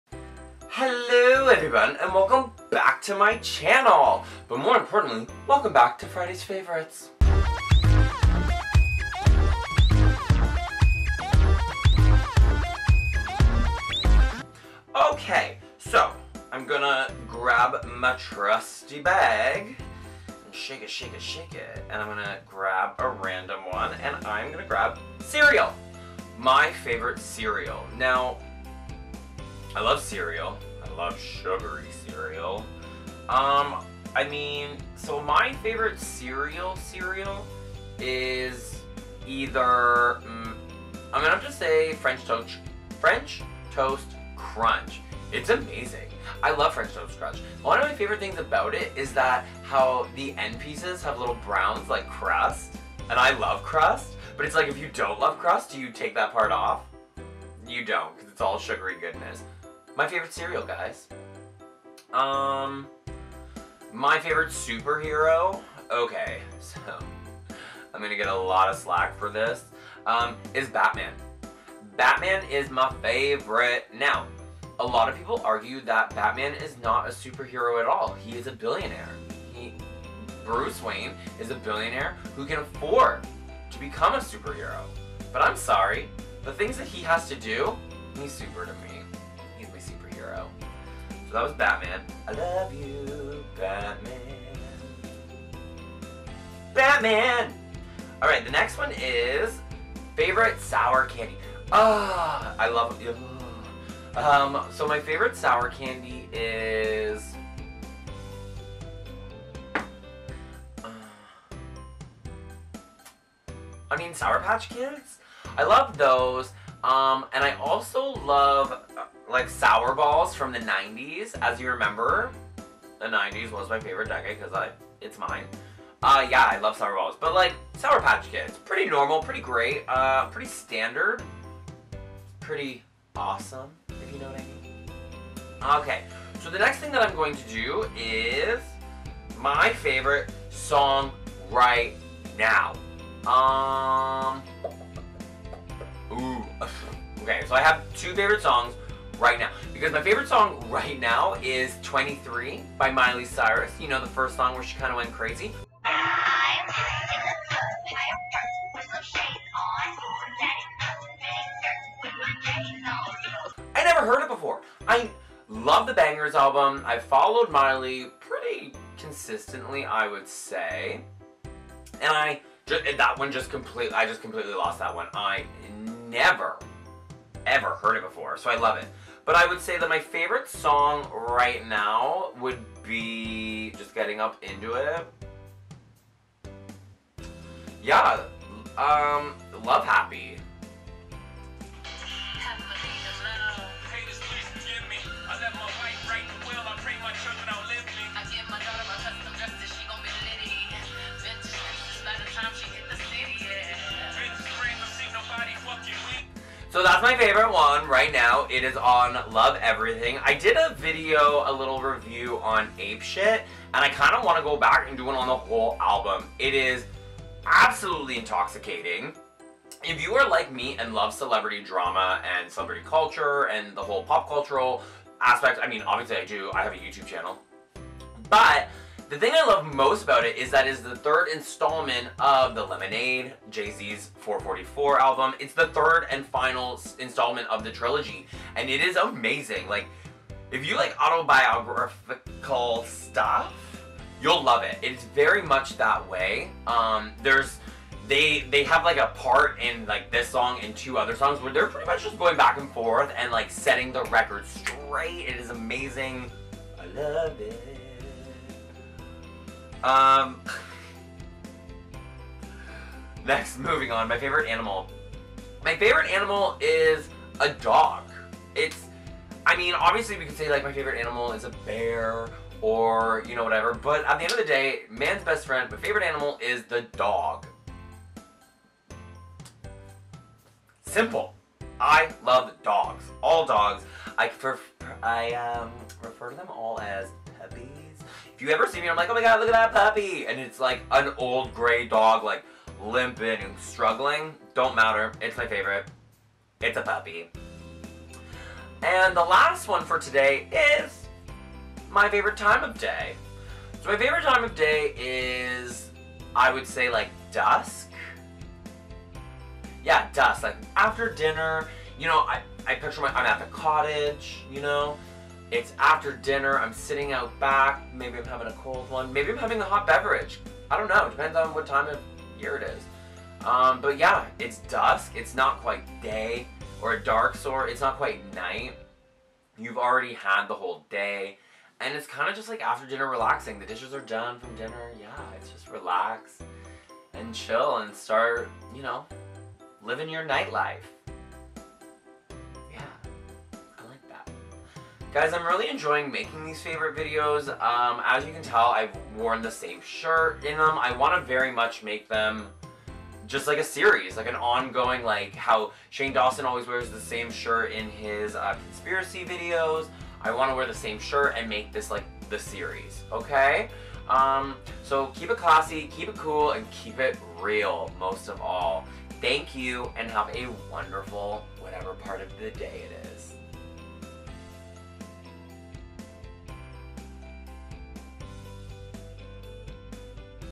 Friday's Favorites! Round 3!
Round 3 of my Friday's Favorites!
Enjoy another round and get to know me a little more! what do I like and how off-topic will I get ???